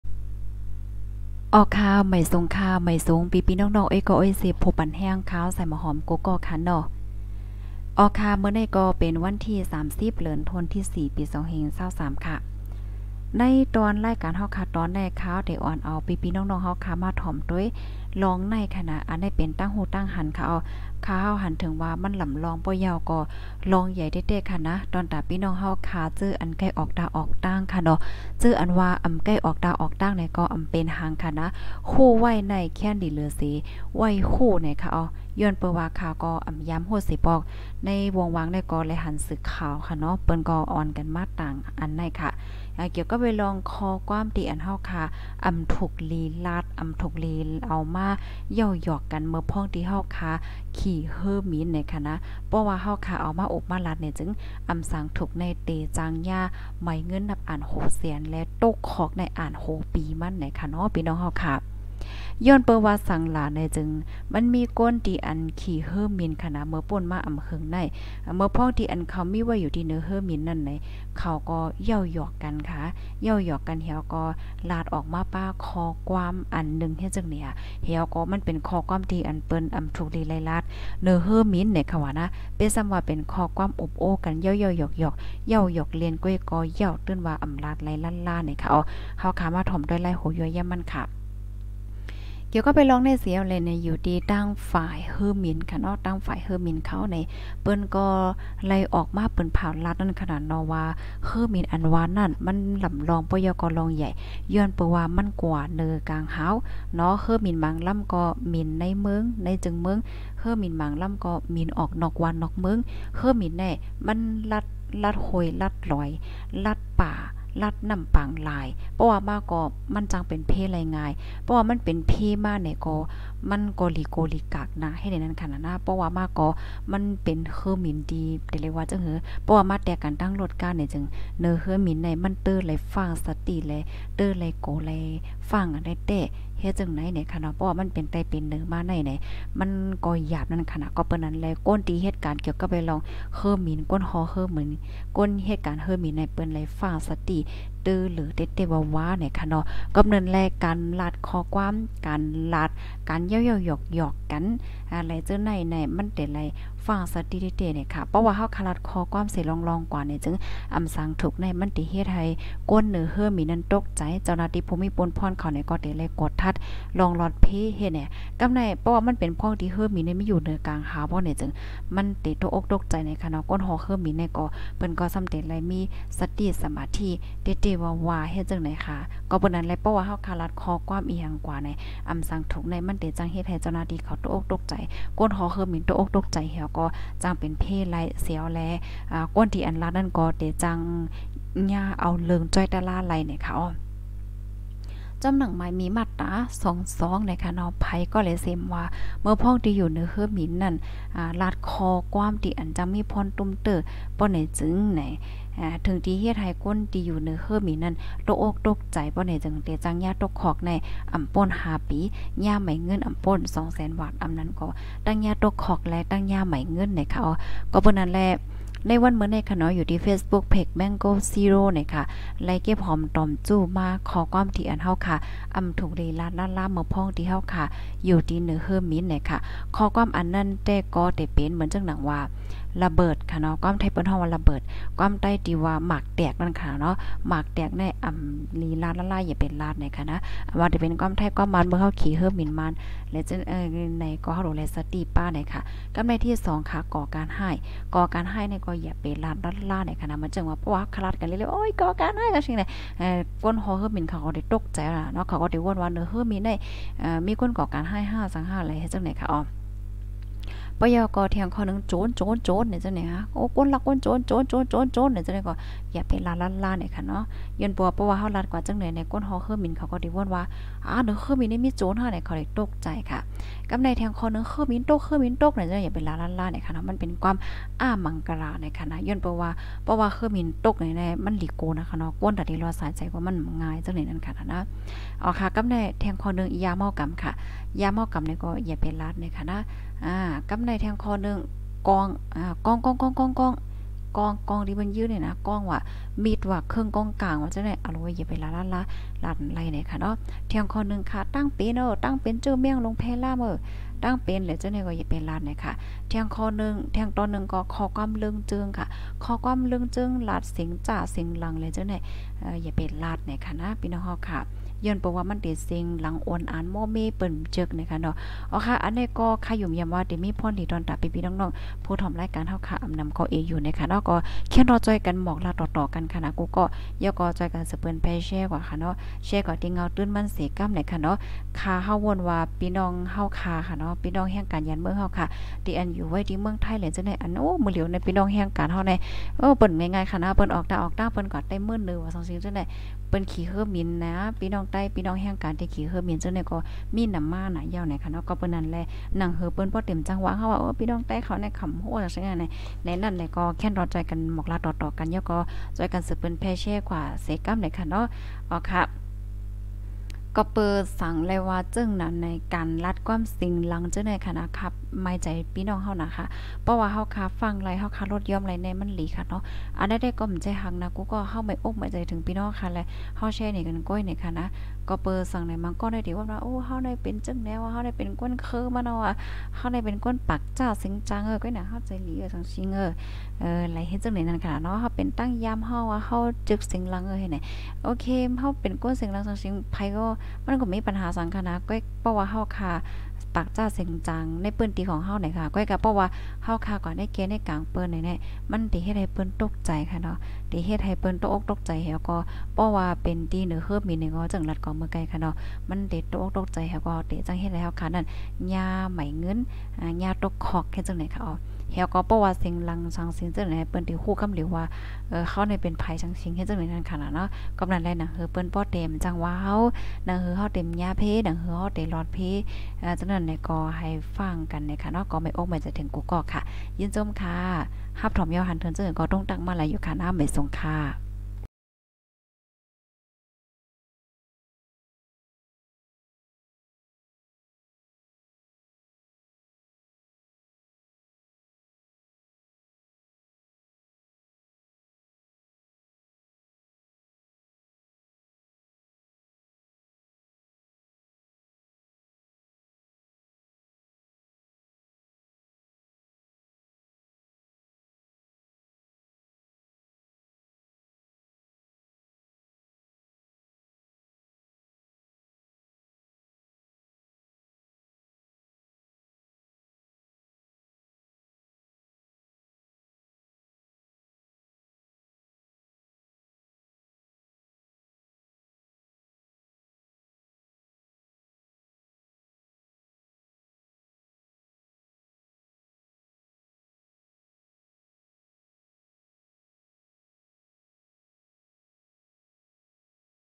ၾၢင့် ! ပေႃးလၢတ်ႈၵႂၢမ်းၸိူဝ်းၼႆ့ၼိူဝ်ႁိူၼ်းမိၼ် ၺႃးတၢမ်ႇၶွၵ်ႇ5ပီလႄႈတၢမ်ႇငိုၼ်းၼပ့်သႅၼ် - 30/4/2023
30/4/2023
Thanks Photo  to  Owner